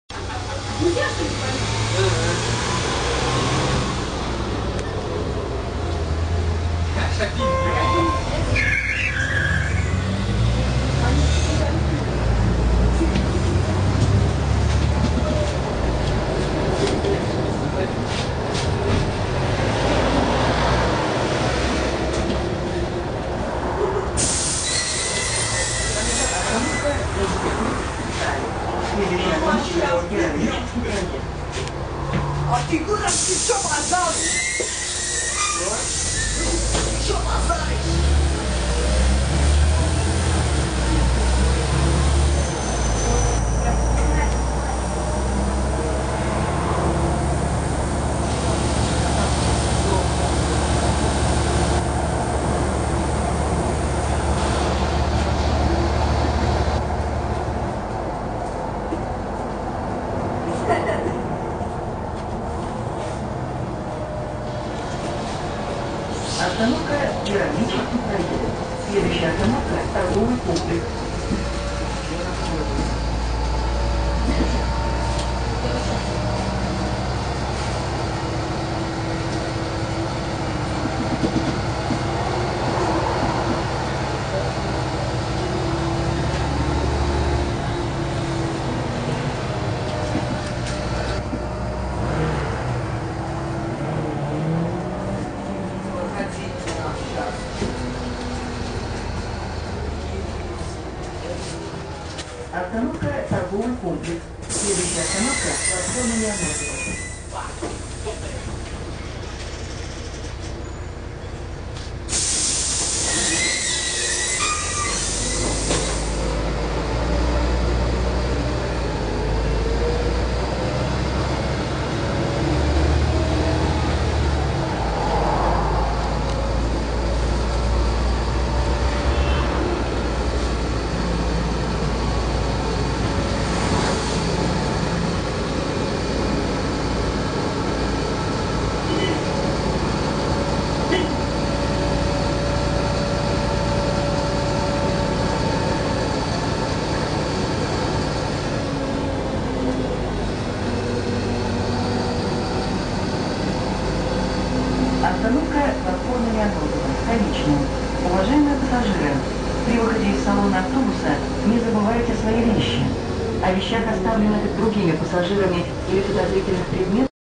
ЛиАЗ 6213 (07382) / LiAZ 6213 in Moscow 18.09.2011
Поездка в автобусе ЛиАЗ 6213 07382. В 7 АП недавно поступил новый ЛиАЗ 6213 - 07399, но сегодня его так и не получилось дождаться. Видео с ним скоро будут!
Еще раз отмечу на всякий случай, что подобные видео сняты для любителей общественного транспорта. Не ищите здесь каких-либо сенсаций, приколов и прочего.